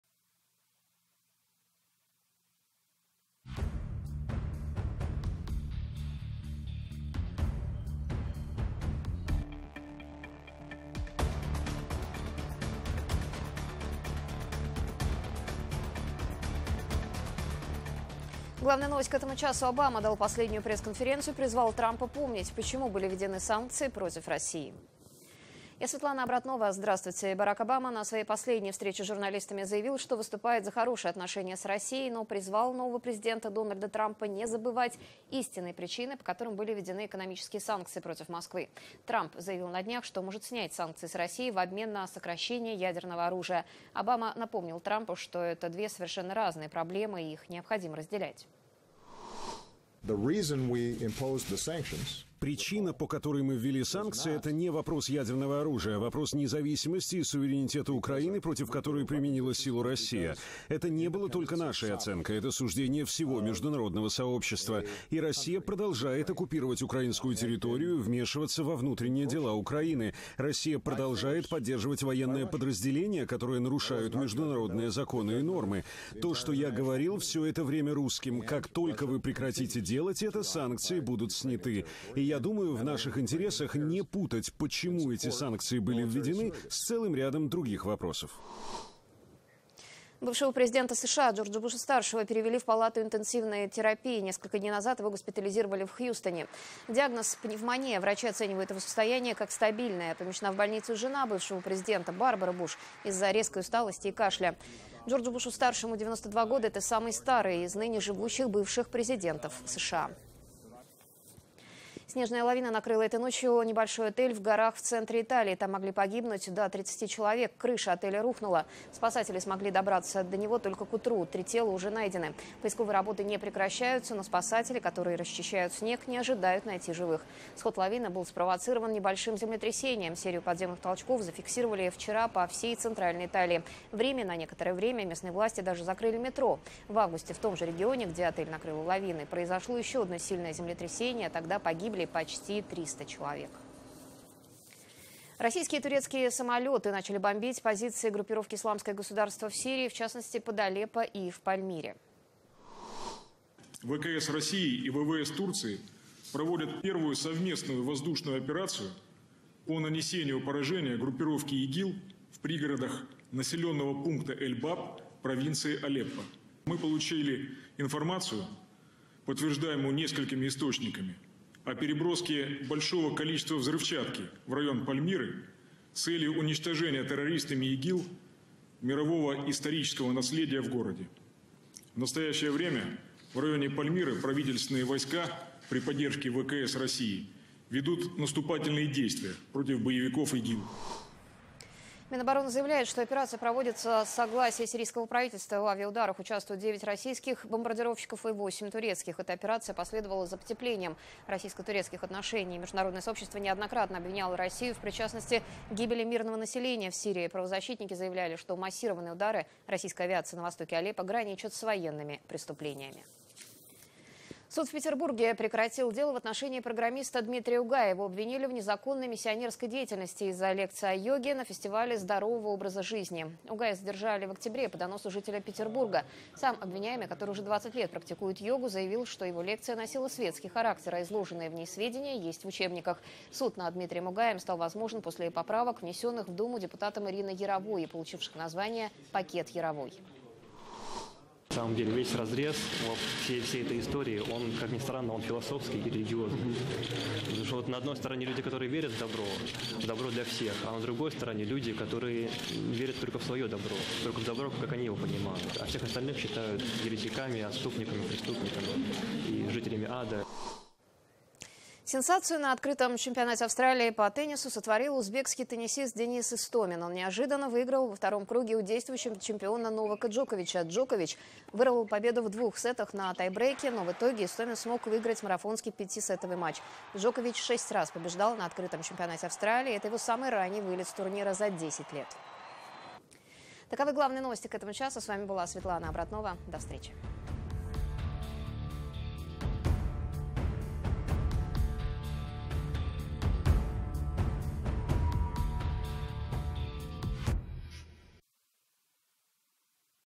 "США не следует забывать о причинах санкций против России"
Президент США Барак Обама заявил, что Вашингтону не следует забывать о том, что санкции против России были им введены после её аннексии Крыма и поддержки сепаратистов на востоке Украины.

Выступая на своей последней в качестве главы государства пресс-конференции, Обама особо подчеркнул, что этот вопрос всегда нужно рассматривать отдельно.

Эти слова прозвучали после того, как на днях заступающий 20 января на пост президента США Дональд Трамп заявил о возможности отменить санкции против России, если она сократит свой ядерный потенциал.

Барак Обама отметил, что за время президентства Владимира Путина в России наблюдается всплеск антиамериканских настроений, что способствует возвращению духа "холодной войны" и осложняет двусторонние отношения.

По словам покидающего свой пост Обамы, конструктивные отношения Вашингтона с Москвой отвечают интересам как Америки, так и всего мира.